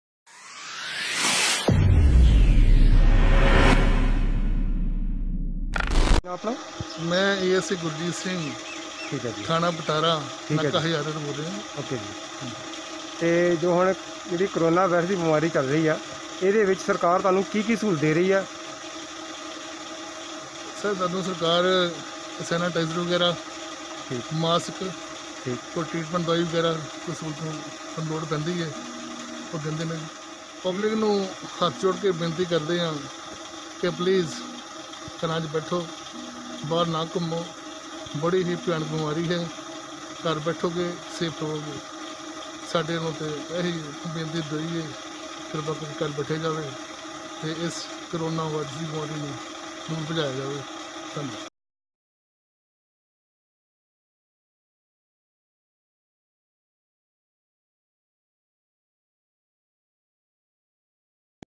Surma Punjab- ਦੇਖੋ ਏ.ਐੱਸ.ਆਈ ਗੁਰਜੀਤ ਸਿੰਘ ਨੇ ਜਨਤਾ ਨੂੰ ਕਿ ਸੰਦੇਸ਼ ਦਿੱਤਾ
POLICE NAKA VPO HAZARA ASI GURJIT SINGH HUMBLE REQUET TO PEOPLE